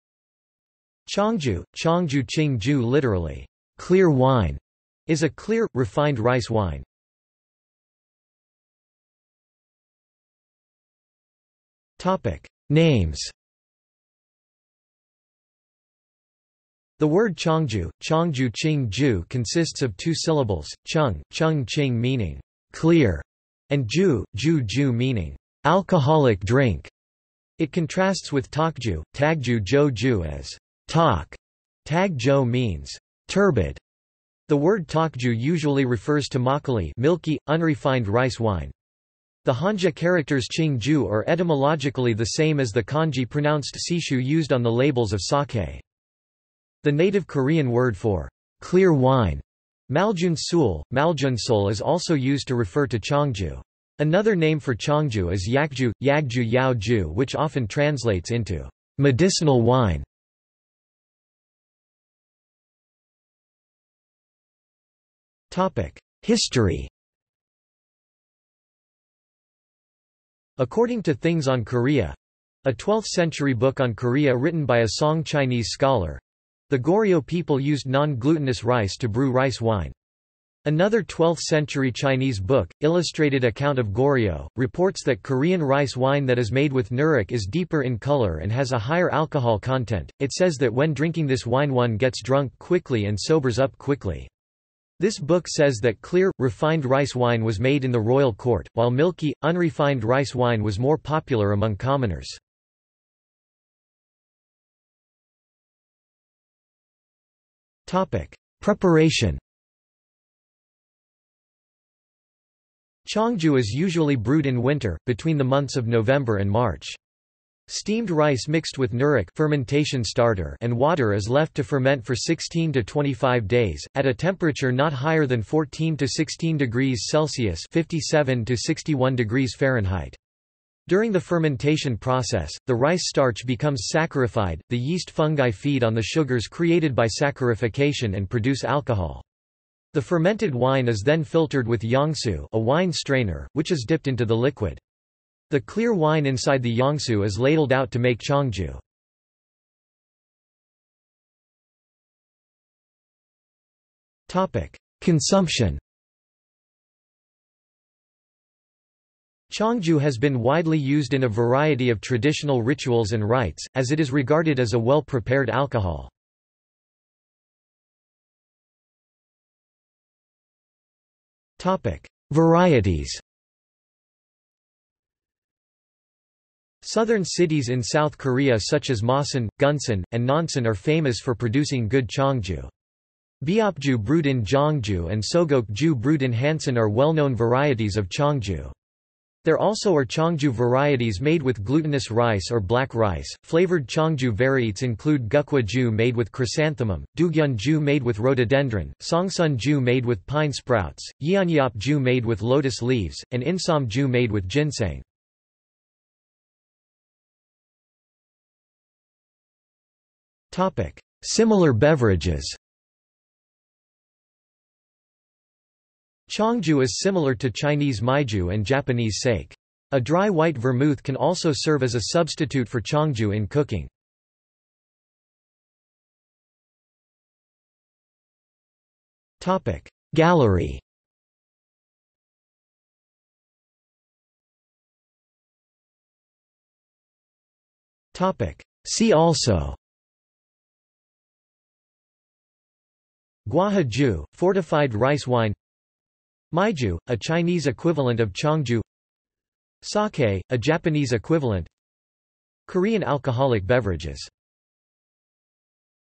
Cheongju (wine) | Wikipedia audio article
This is an audio version of the Wikipedia Article:
Cheongju (wine)

00:00:08 1 Names
00:01:01 2 History
00:01:44 3 Preparation
00:02:30 4 Consumption
00:02:48 5 Varieties
00:03:26 6 Similar beverages
00:03:45 7 Gallery
00:03:54 8 See also

- Socrates

SUMMARY
Cheongju (청주; 淸酒; literally "clear wine") is a clear, refined rice wine.